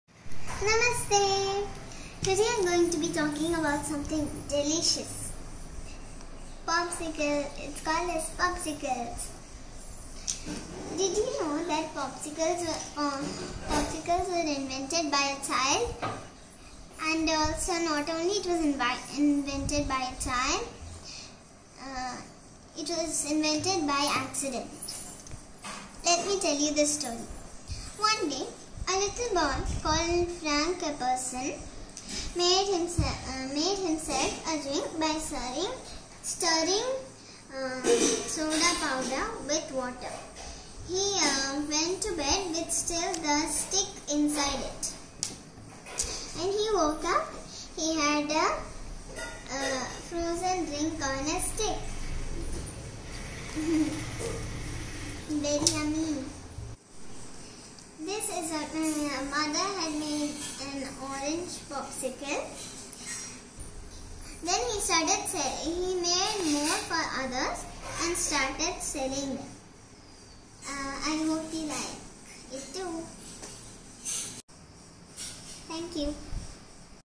All about Posicles
Avani shares more about how popsicle was an accidental invention by a small boy. This was her part of her summer assignment and she loved it.